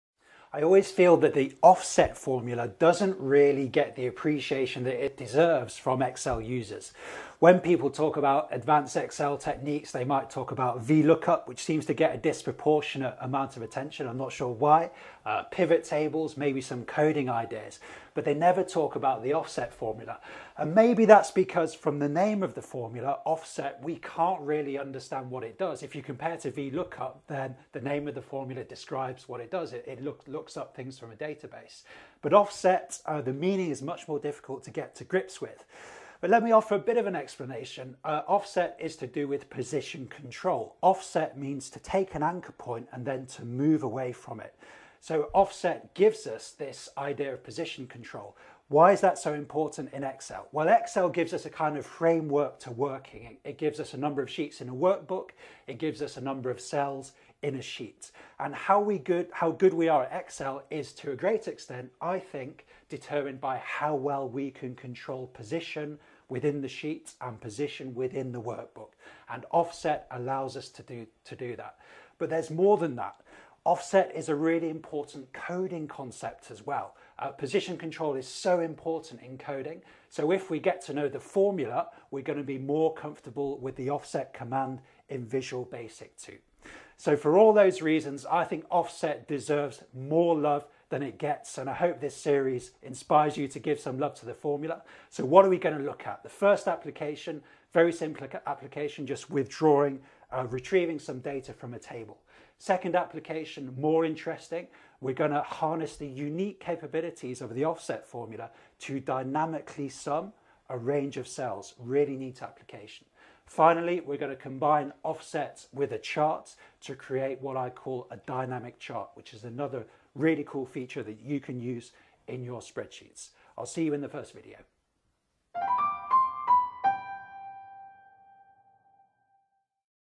Excel Offset Function Tutorial 1/4
What is the Excel offset function, and how does the Excel offset function work?

In  this video series, we explore the unique capabilities of the Excel offset function, and look at three applications in Excel.

The Excel Offset Function is an awesome Excel formula with unique capabilities. Yet, the Excel Offset Function receives little attention from Excel users, and is overshadowed by its more glamorous cousins - the LOOKUP functions. Why is this? The aim of this series is to explore this issue, to shed light on the capabilities of the function, and to inspire you to use it in your work. So go on, give it some love!

I would love to hear from you – did the series help, and what are you trying to get done in Excel?

For regular spreadsheet hints and tips and more on the ExcelRevolution